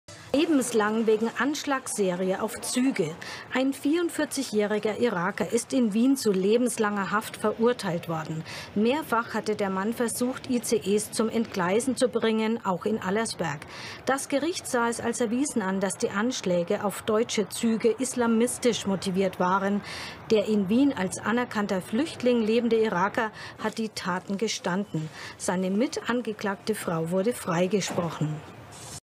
Anschläge auf ICE-Züge: Lebenslange Haft für irakischen Flüchtling
Nach einer islamistisch motivierten Anschlagsserie auf ICE-Züge in Deutschland 2018, ist ein 44-jähriger Iraker zu lebenslanger Haft verurteilt worden.